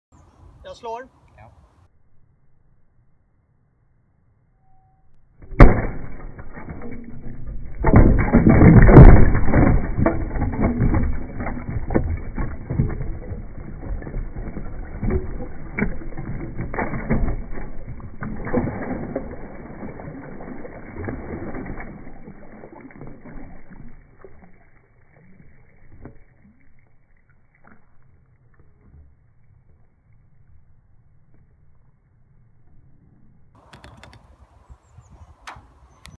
Ice Chunk Shattered by Sledgehammer in Slow Motion
Shot @ 240 FPS with 720p resolution. Playback at 30 FPS.

For further inquiries tweet to me at: @AlexLandherr

(my profile picture is of a SpaceX Dragon capsule)